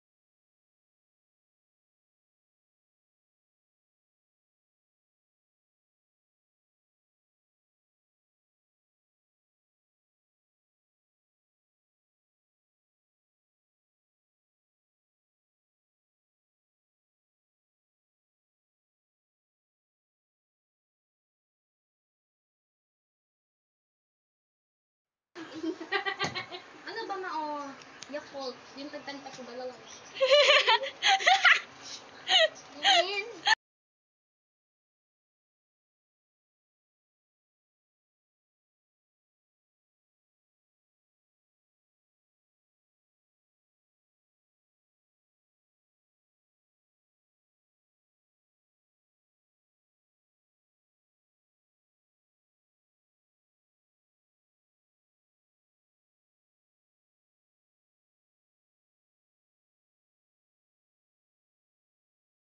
My angel birthday 9th birthday simple celebration//simple Lhou
Appreciate of what we have, simple celebration of my daughter 9th birhtday during pandemic time.. Thank you God ❤️🙏.. I love you so much glaiza faith you are always my little angel what ever happened, kahit na tatanda kana♥️..